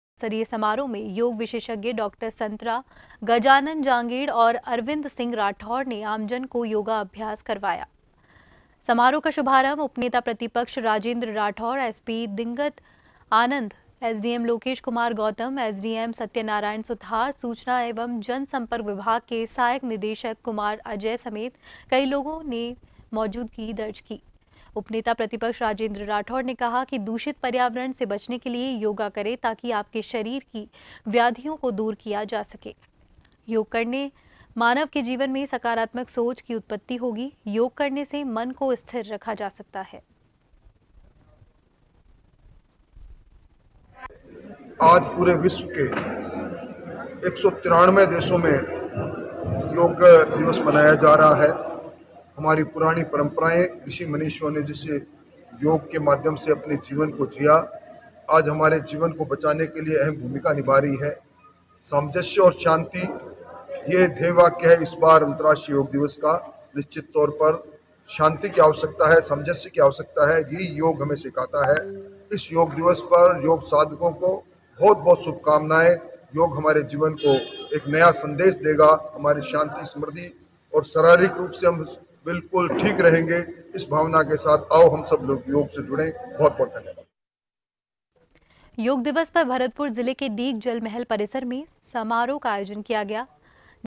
चूरू- अंतराष्ट्रीय योग दिवस पर योग शिविर,जिलास्तरीय योग शिविर में किया योग
चूरू- अंतराष्ट्रीय योग दिवस पर योग शिविर,
जिलास्तरीय योग शिविर में किया योग

एंकर - चूरू के पुलिस लाइन में आयुर्वेद विभाग एंव जिला प्रशासन की ओर से अंतर्राष्ट्रीय योग दिवस पर आयोजित जिला स्तरीय समारोह में योग विशेषज्ञ डॉ संतरा, गजानन्द जांगिड़ व अरविन्द सिंह राठौड़ ने आमजन को योगाभ्यास करवाया। समारोह का शुभारंभ उपनेता प्रतिपक्ष राजेन्द्र राठौड़, एसपी दिगंत आनन्द, एडीएम लोकेश कुमार गौतम, एसडीएम सत्यनारायण सुथार, सूचना एंव  जनसंपर्क विभाग के सहायक निदेशक कुमार अजय, पूर्व जिला प्रमुख हरलाल सहारण, सिटी सीओ राजेन्द्र बुरडक, आदि ने माँ सरस्वती के आगे दिप प्रज्ज्वलित कर किया।  उपनेता प्रतिपक्ष राजेन्द्र राठौड़ ने कहा कि दूषित पर्यावरण से बचने के लिए योगा करे। ताकि आपके शरीर की व्याधियों को दूर किया जा सके। योग करने मानव के जीवन मे सकारात्मक सोच की उत्पत्ति होगी। योग करने से मन को स्थिर रखा जा सकता है।  आयुर्वेदिक विभाग से उपनिदेशक डॉ अनिल मिश्रा, डॉ रामकृष्ण शर्मा, डॉ संजय तंवर, डॉ कमल वशिष्ठ, डॉ भालेन्दु, डॉ मनीष, डॉ योगेंद्र, डॉ अहसान गौरी व डॉ कपिल वर्मा आदि ने आयोजिकीय भूमिका निभाई।

बाइट- राजेन्द्र राठौड़, चूरू विधायक।

रिपोर्ट — कौशल शर्मा
चूरू